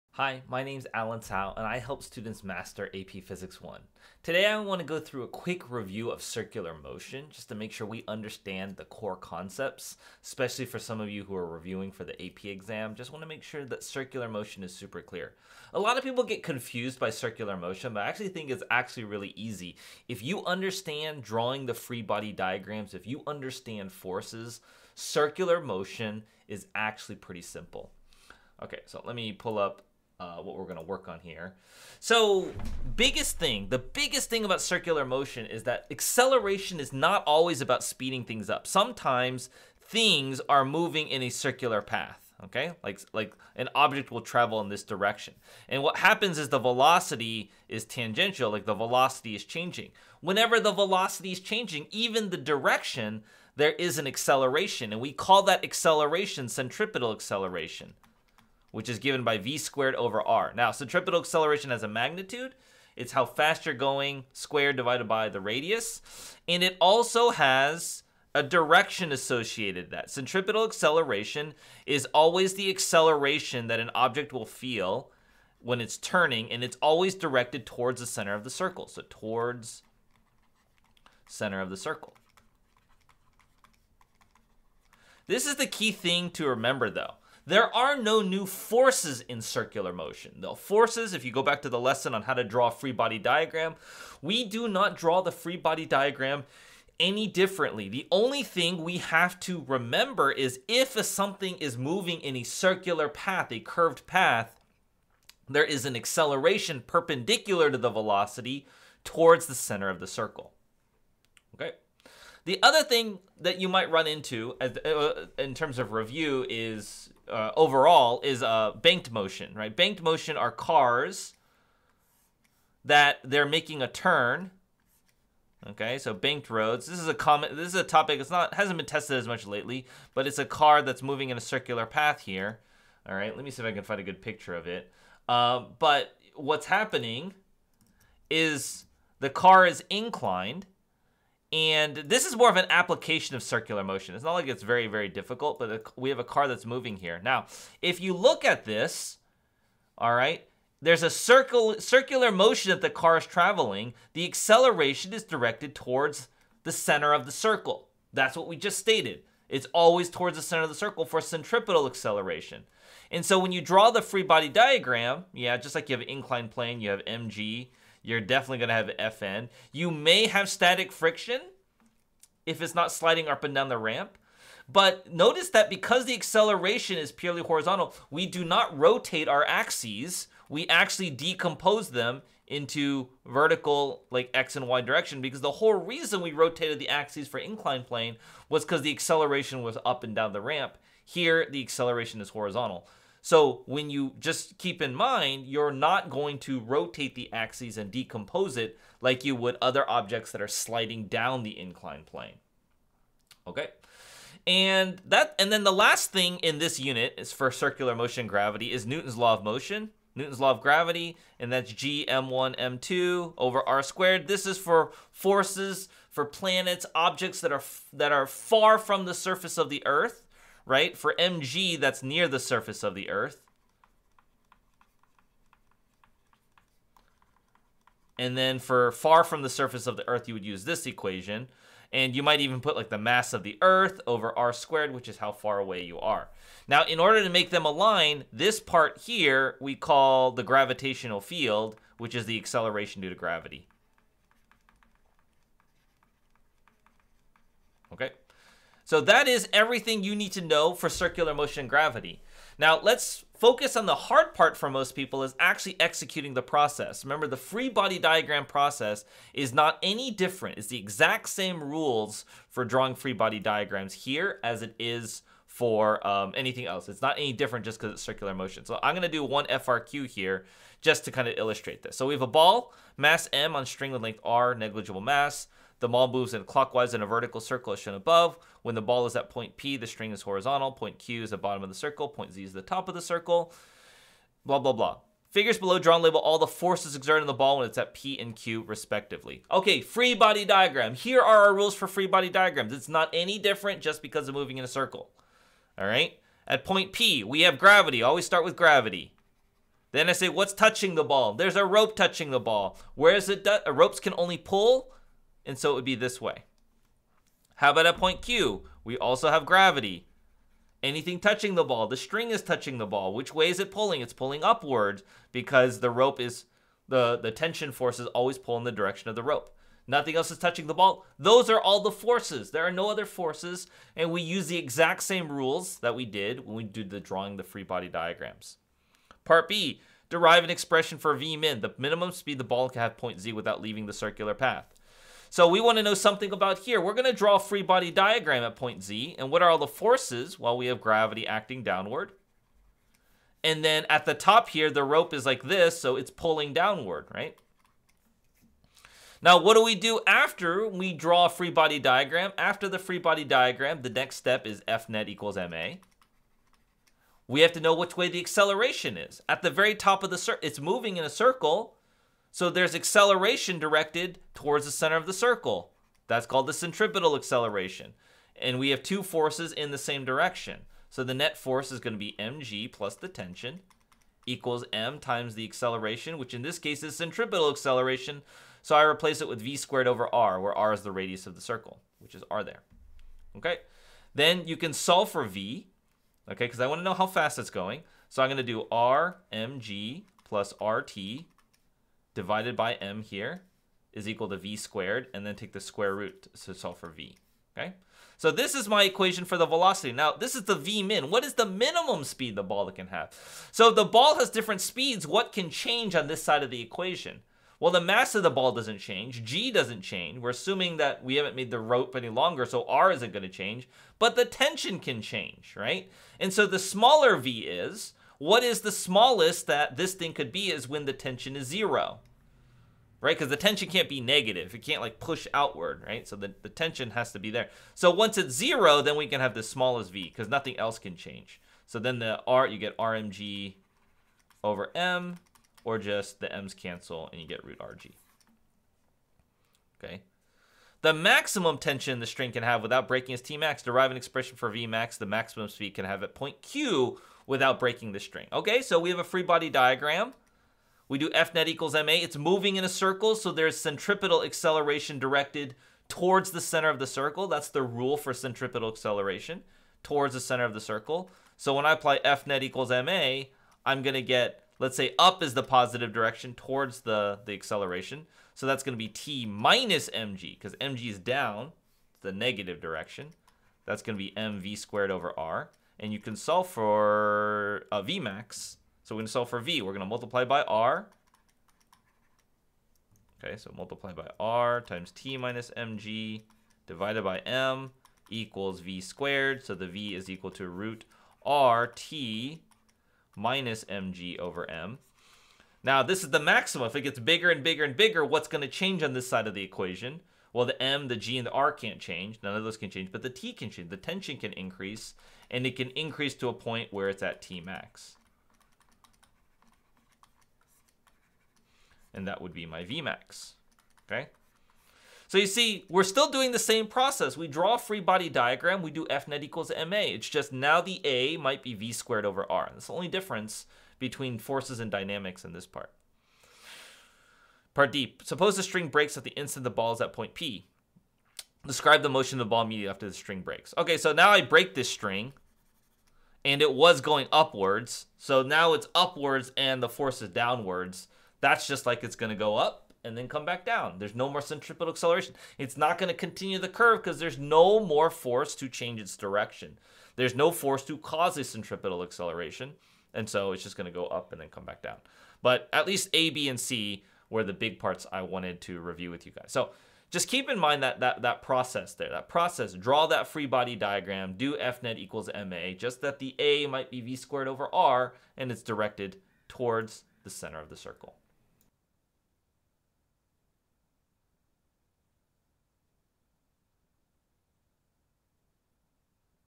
Episode 21 - 10 Minute AP Physics 1 Circular Motion Review
A lot of students find circular motion to be confusing.

But it’s actually pretty simple if you understand your forces and free body diagrams.

Take a look at this 10-minute circular motion review for AP Physics 1.

See how my AP® Physics 1 Secrets of Problem Solving has been helping hundreds of students master physics.